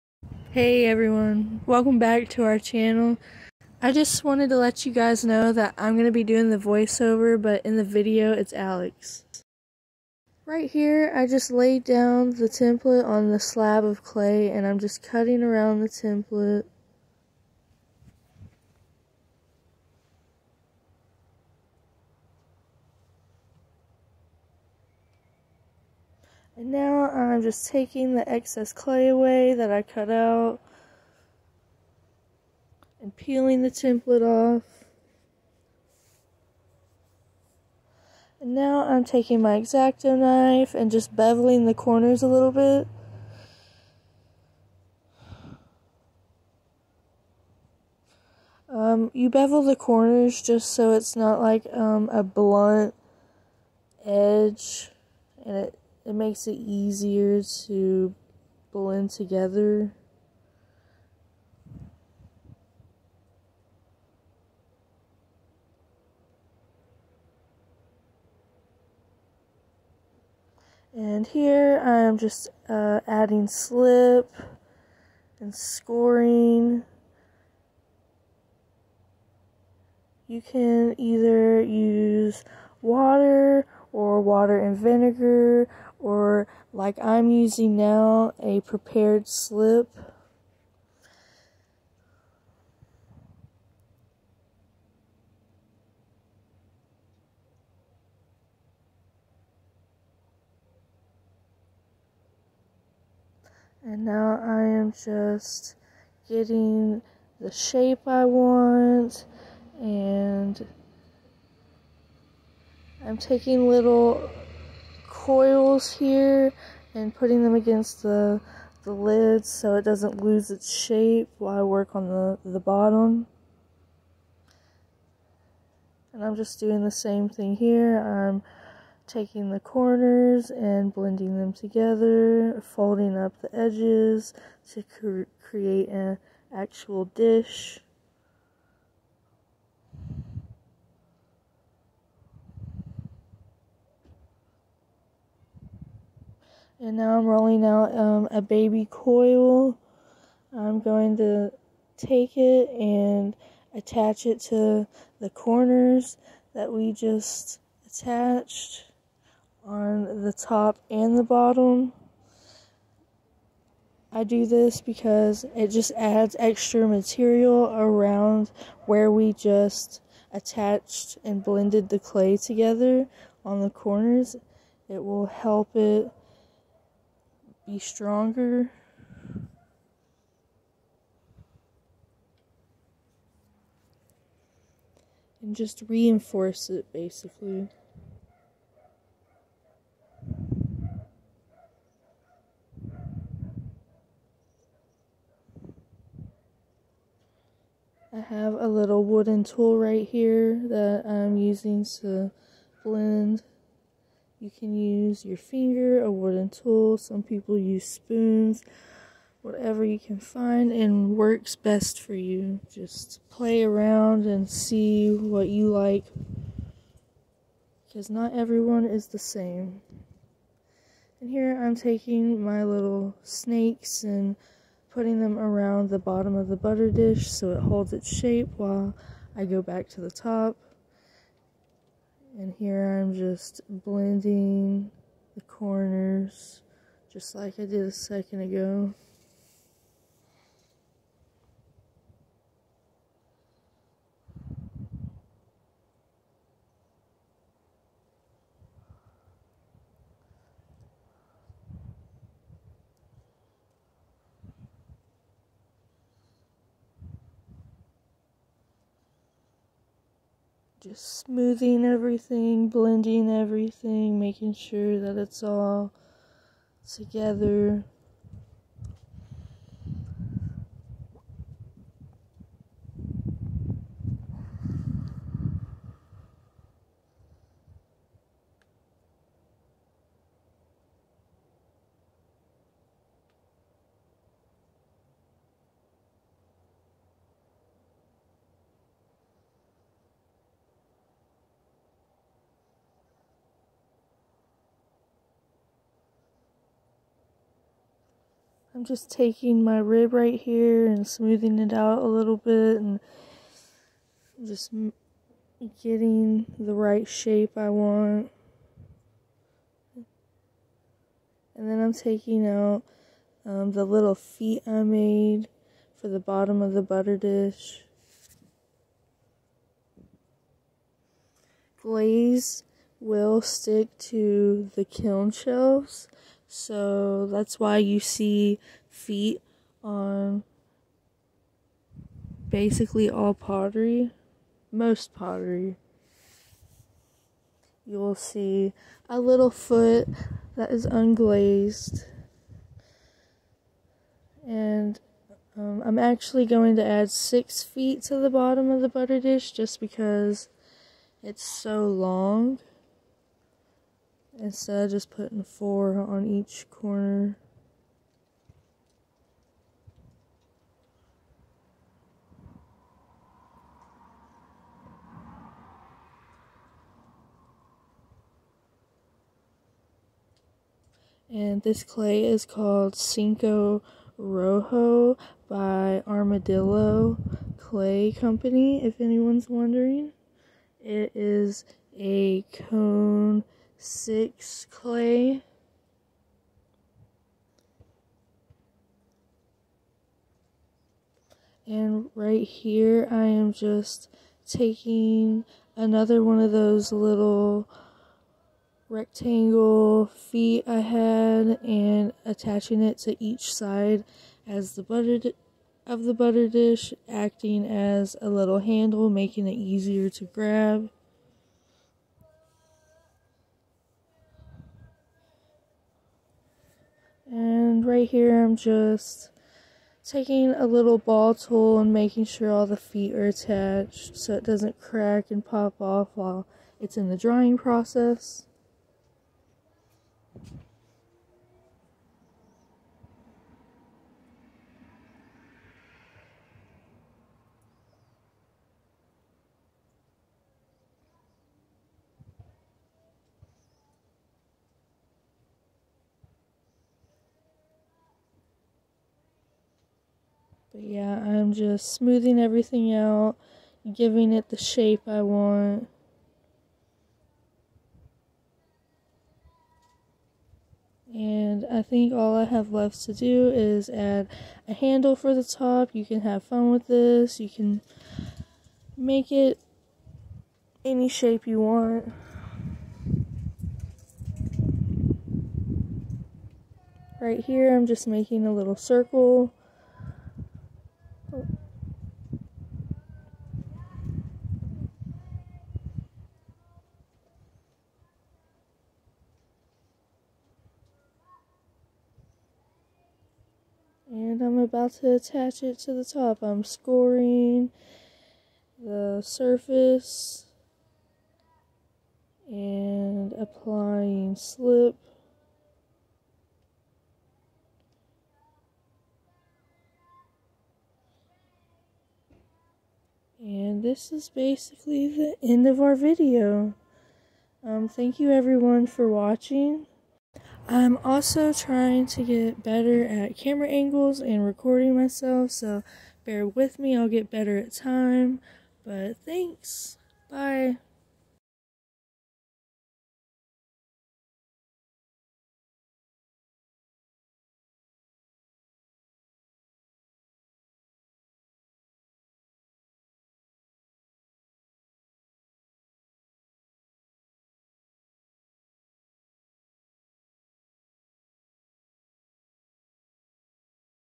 making a ceramic butter dish at home
hey! everyone this is Kalli and Alex here just playing with clay and making ceramics at home please enjoy our first voice over video and please bear with me while I get better at filming and editing!